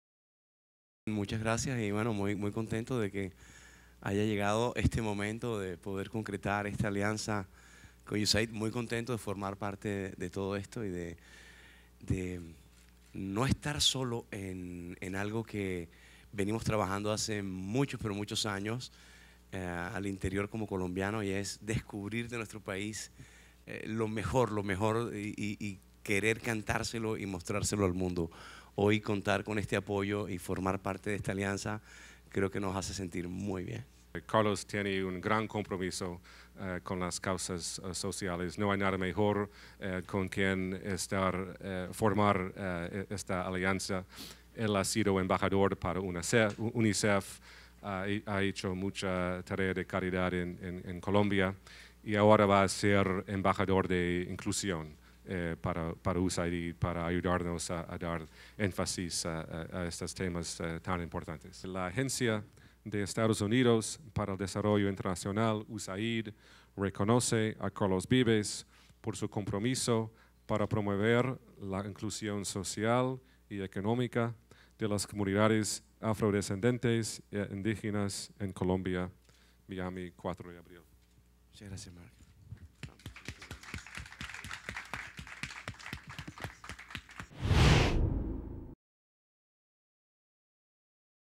Carlos Vives apoya a la USAID en Estados Unidos
El cantautor  colombiano Carlos  Vives es nuevo  embajador de la agencia de los Estados Unidos  para el Desarrollo Internacional, Usaid, a través de su programa  para Afrodecendientes.